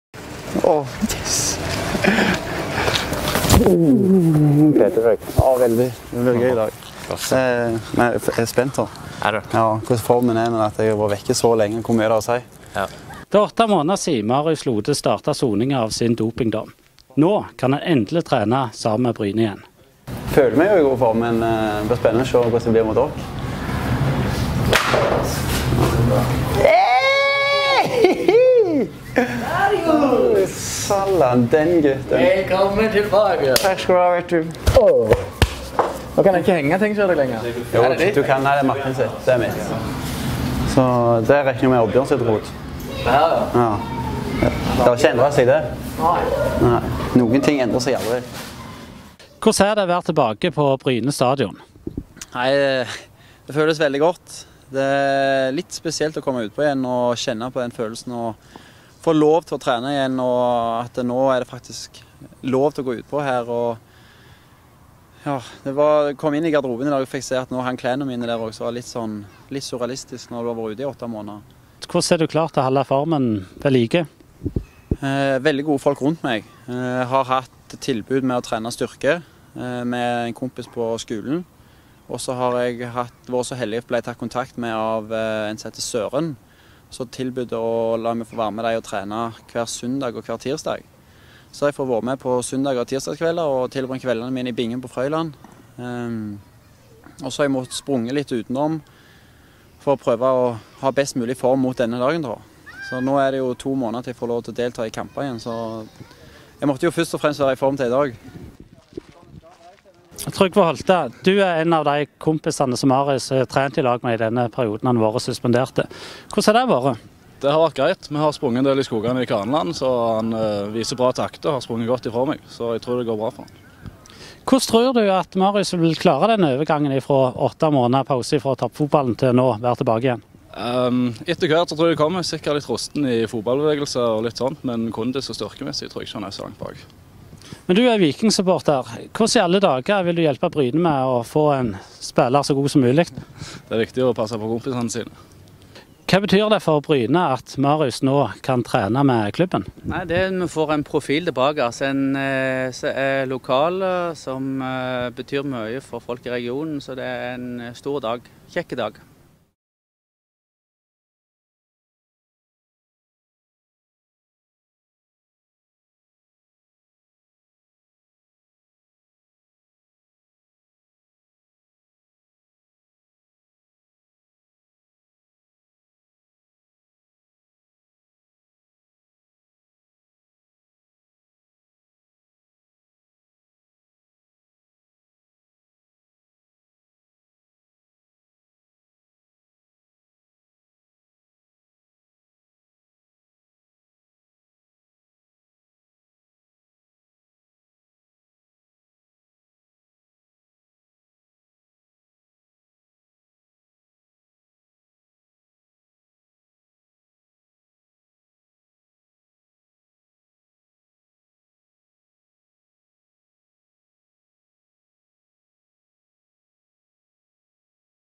Stor glede då Marius Lode kunne trena med Bryne igjen
Marius Lode hadde si første trening med Bryne etter å ha sona dopingdommen på åtte månadar.